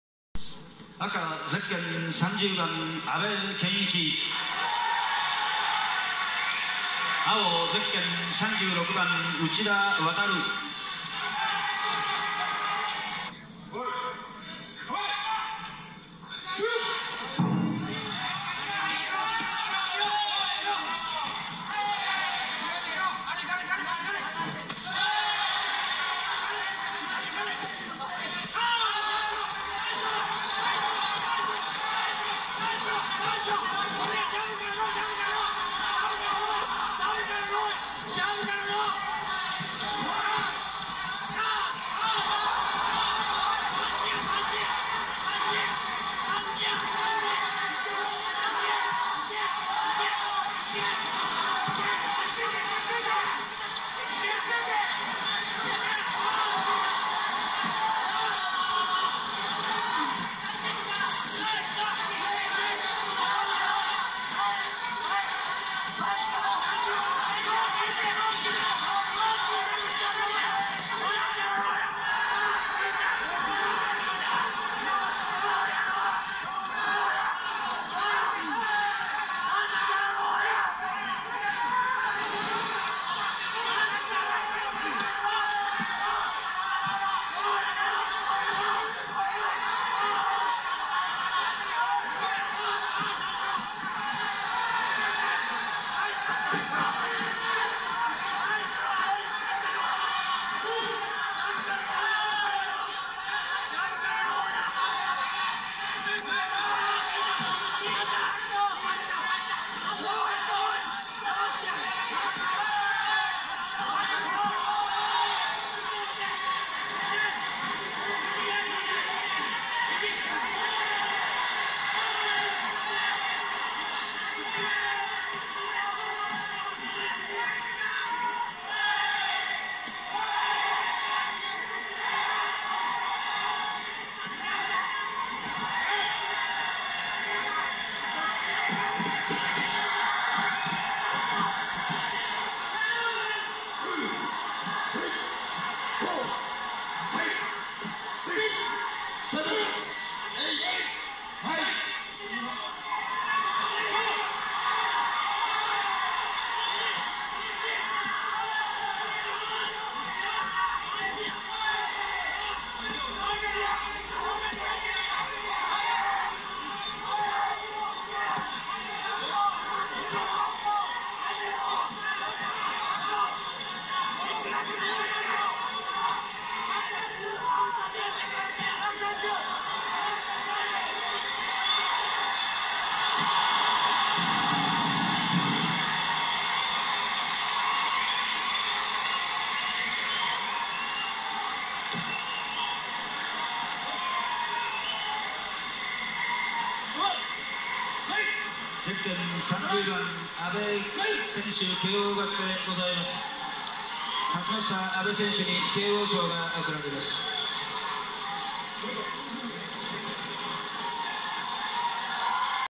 内田弥vs阿部健一 体重差33Kg Uchida Wataru vs Kenichi Abe weight difference 73lb
トーワ杯カラテジャパンオープントーナメント 1992年1月12日 東京武道館